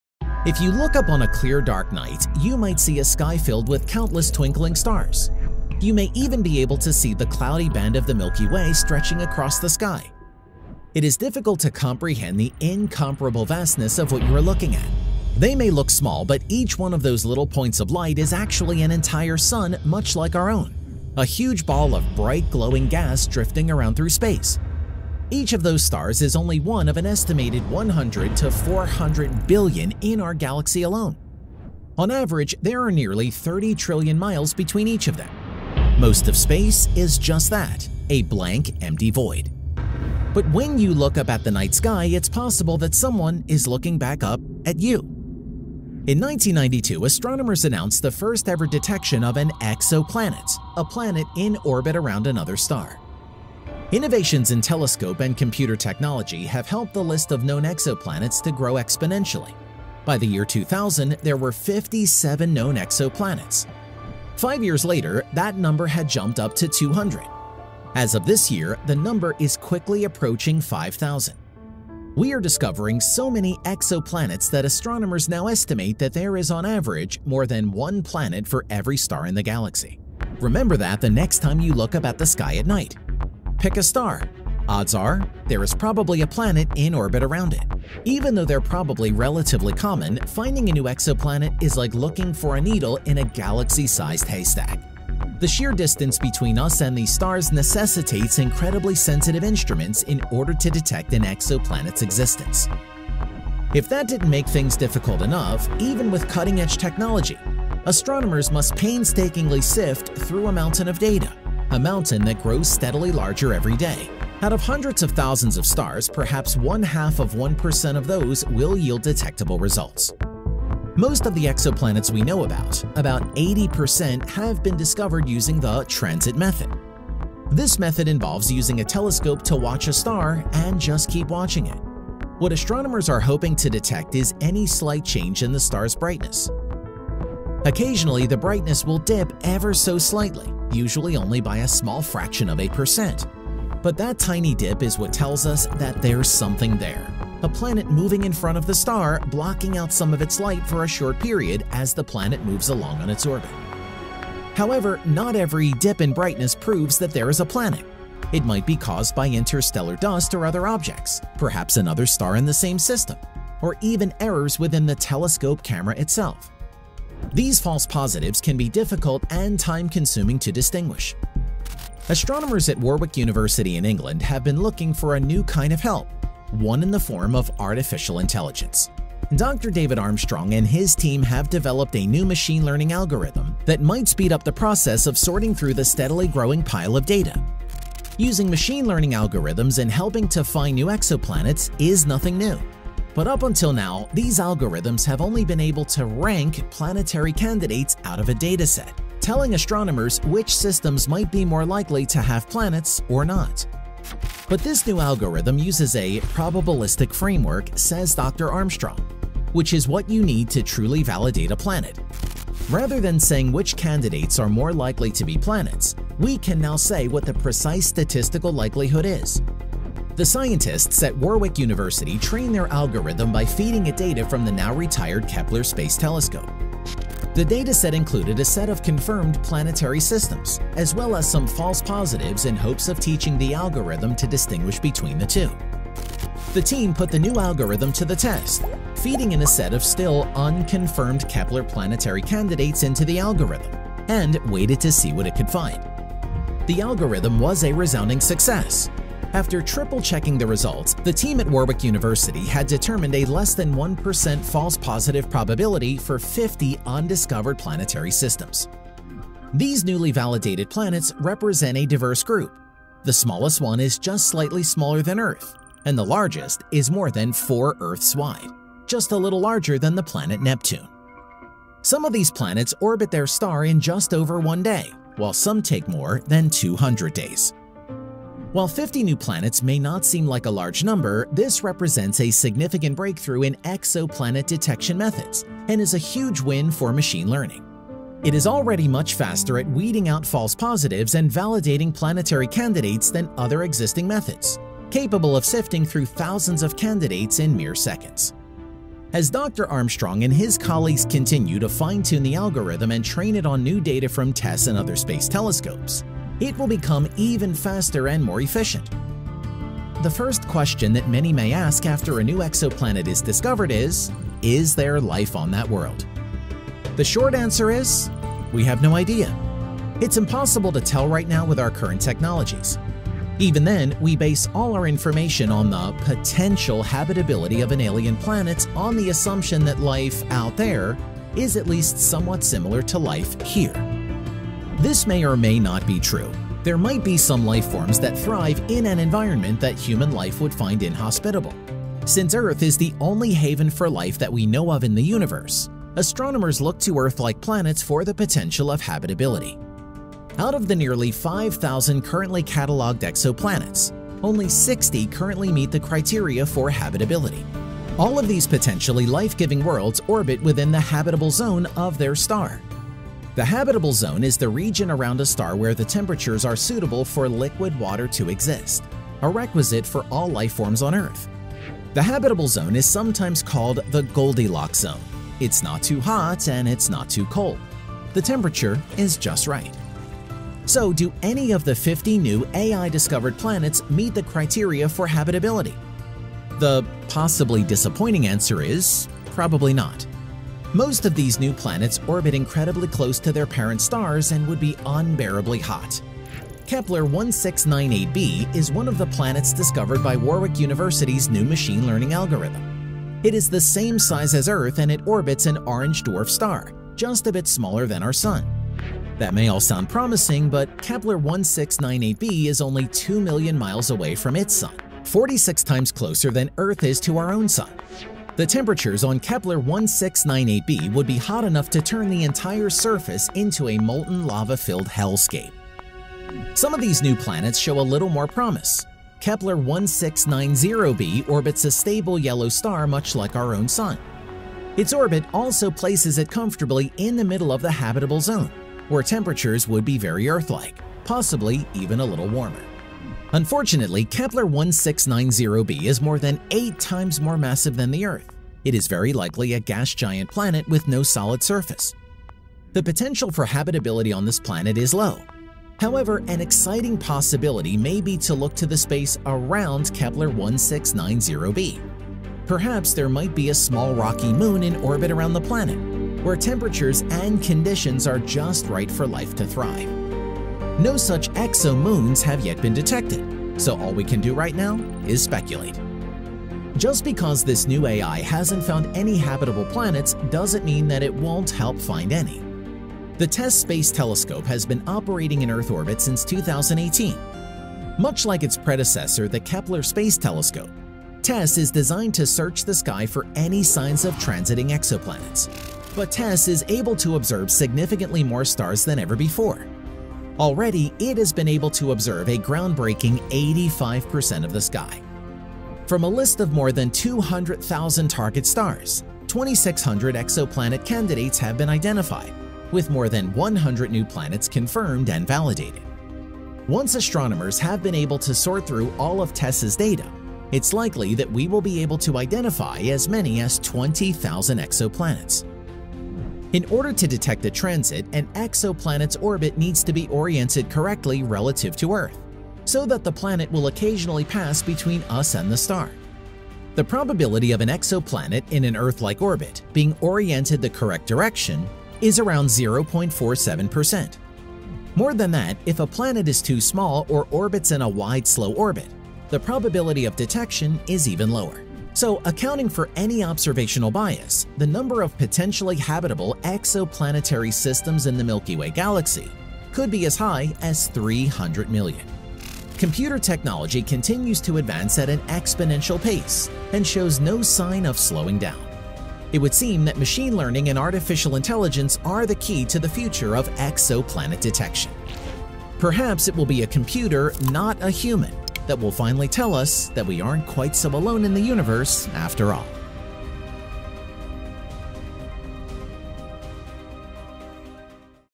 How AI Found 50 Exoplanets
Traditionally, astronomers would manually search for exoplanets but in today's day and age, artificial intelligence is used to find these new planets. An exoplanet is a planet that orbits a star outside of our solar system. Astronomers from all over the world have been spending lots of time using these new AI methods to make new exoplanet discoveries.

Enjoy today's episode Exoplanets: How Artificial Intelligence Found 50 of Them

Topics Discussed/Related:
- artificial intelligence exoplanets
- how artificial intelligence finds exoplanets
- ai exoplanets